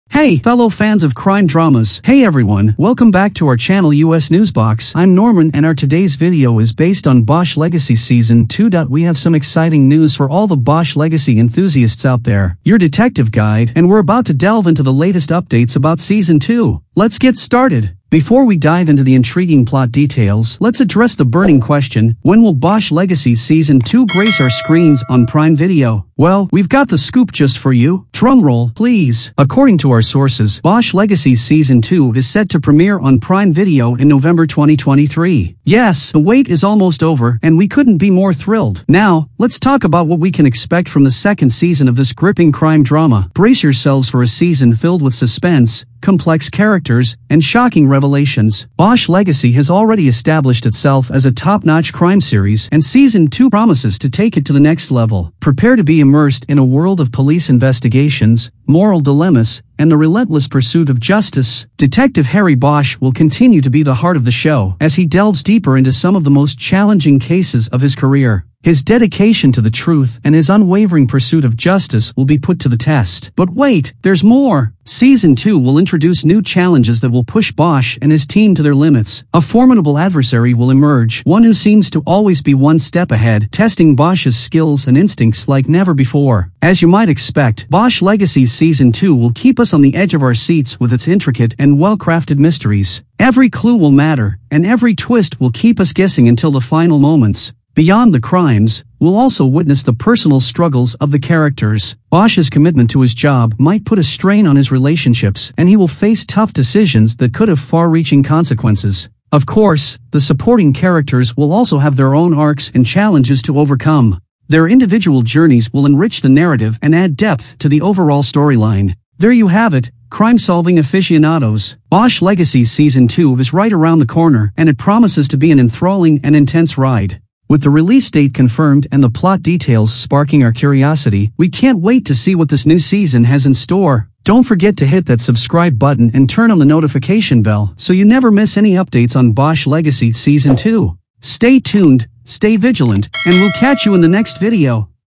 Bosch Legacy Season 2: Premiere Date Unveiled and Intriguing Plot Teased- US News Box Official
Bosch Legacy Season 2: Premiere Date Unveiled and Intriguing Plot Teased

we have some exciting news for all the "Bosch Legacy" enthusiasts out there. your detective guide, and we're about to delve into the latest updates about Season 2. Let's get started!
Before we dive into the intriguing plot details, let's address the burning question: When will "Bosch Legacy" Season 2 grace our screens on Prime Video? Well, we've got the scoop just for you.